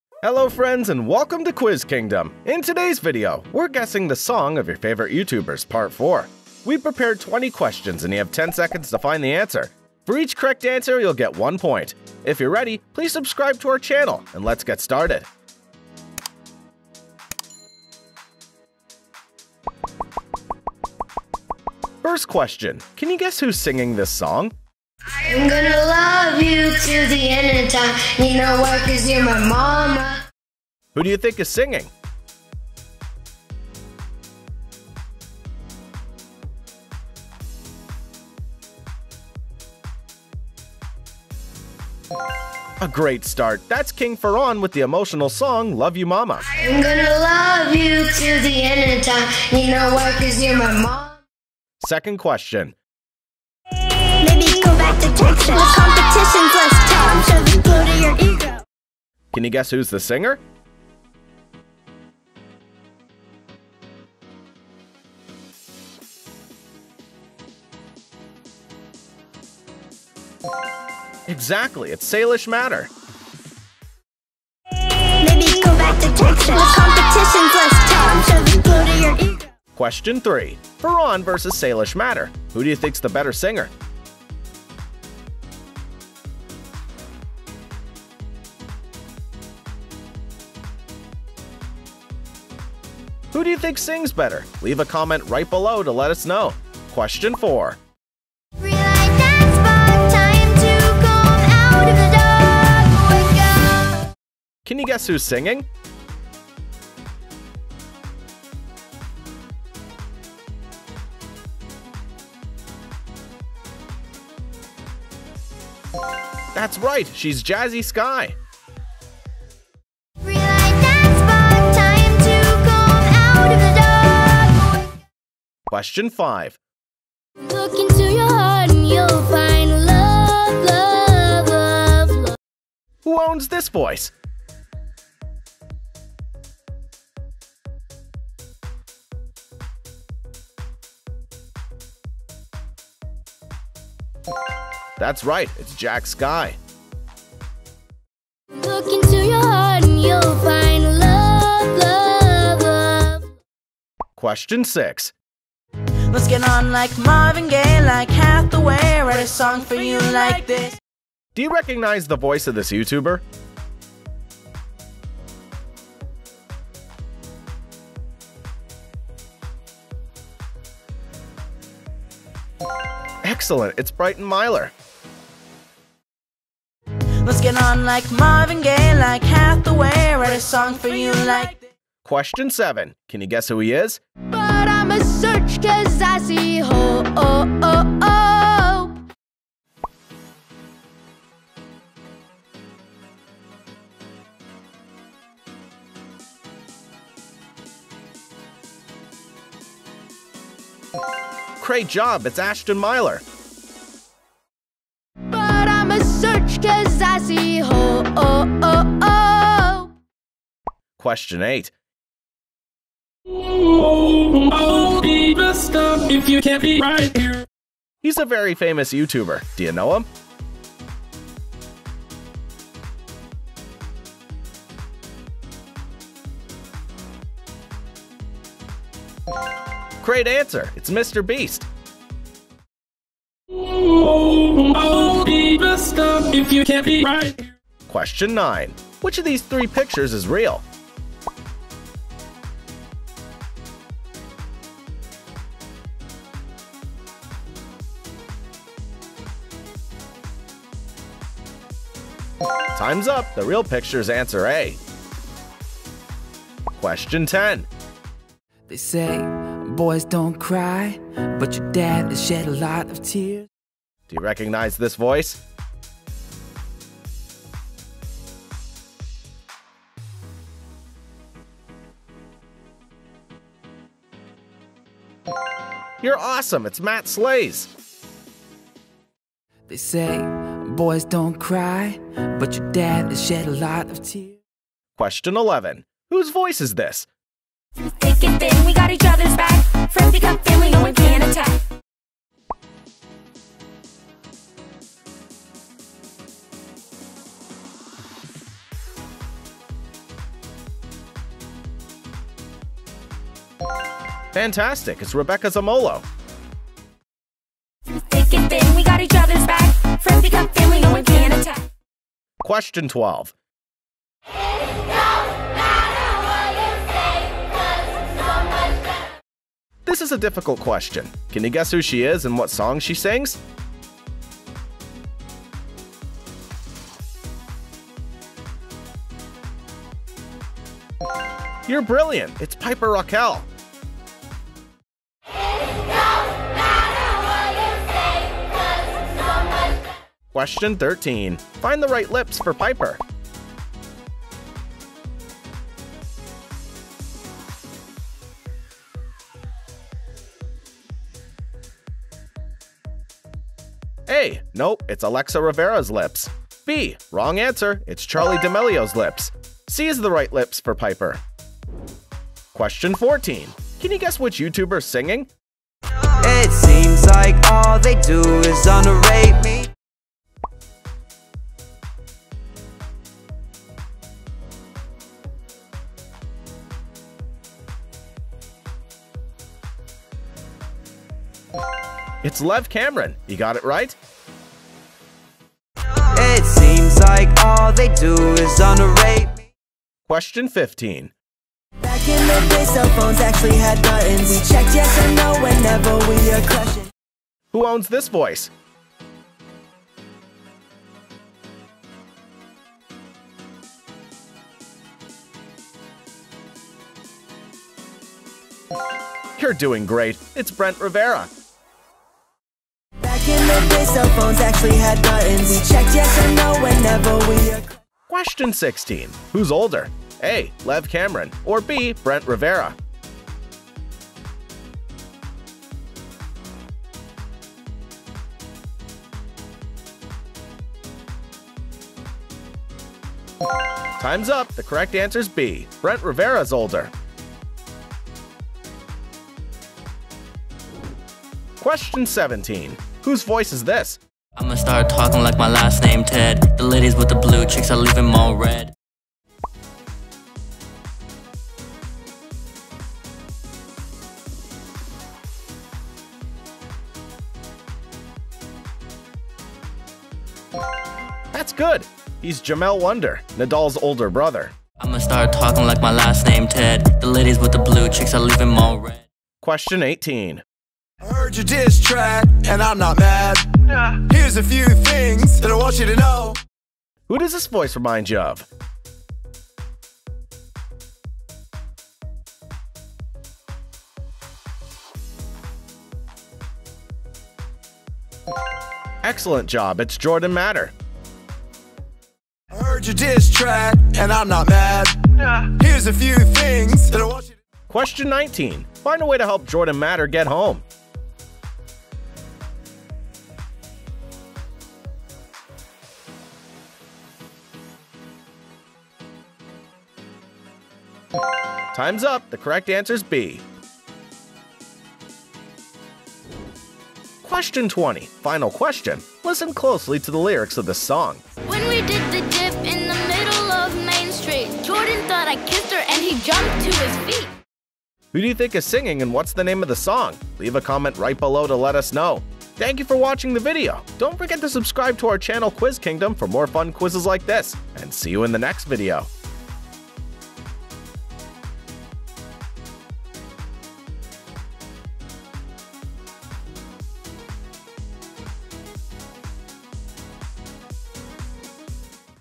Guess The SONG of Your Favorite Youtubers #4 | Royalty Family, Salish Matter, MrBeast, Ninja Kidz TV
Can You Guess The SONG of Your Favorite Youtubers #4 | Royalty Family, Salish Matter, MrBeast, Ninja Kidz TV
We hope you enjoy guessing the Youtuber by their Songs.